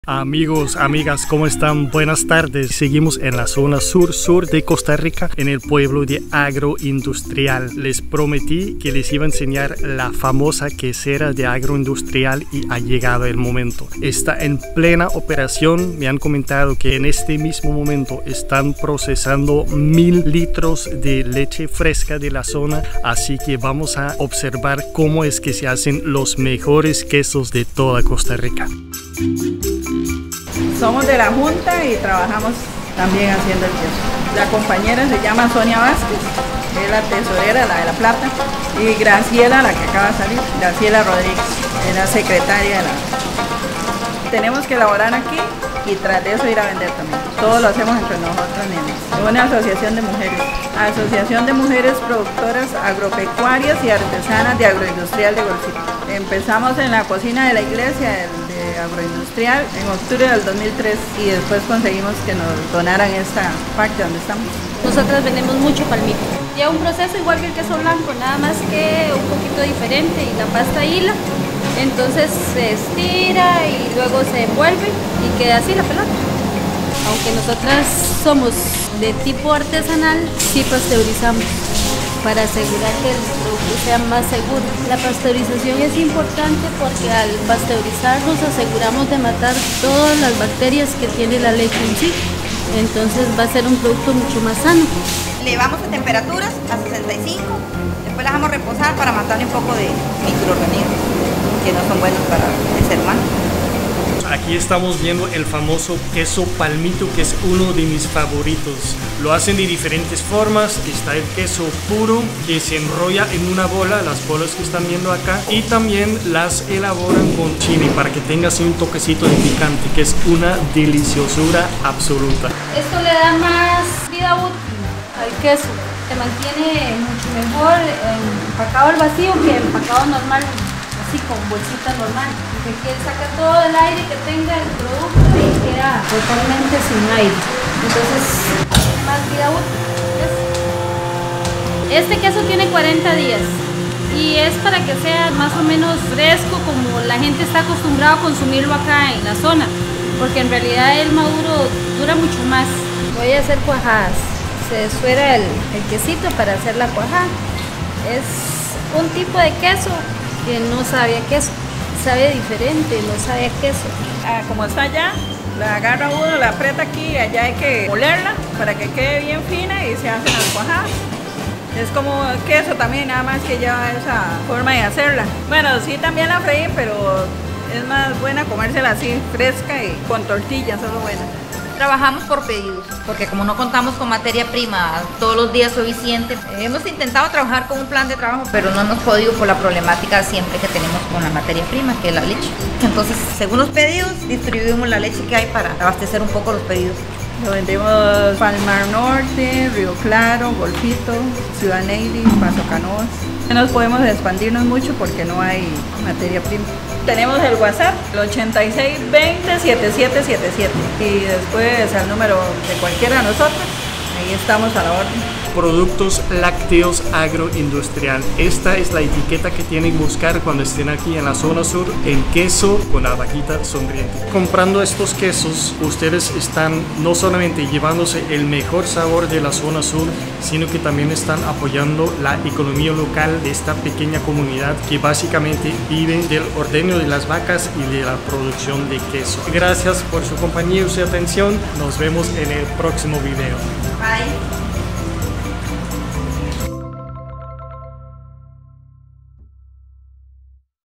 Costa Rica, Agroindustrial
Agroindustrial, a tiny hamlet of 15 people and 200 cows described by Google Maps as a "ghost town," turns out one of the finest products you'll find in Costa Rica, thanks to a courageous team of strong-willed women.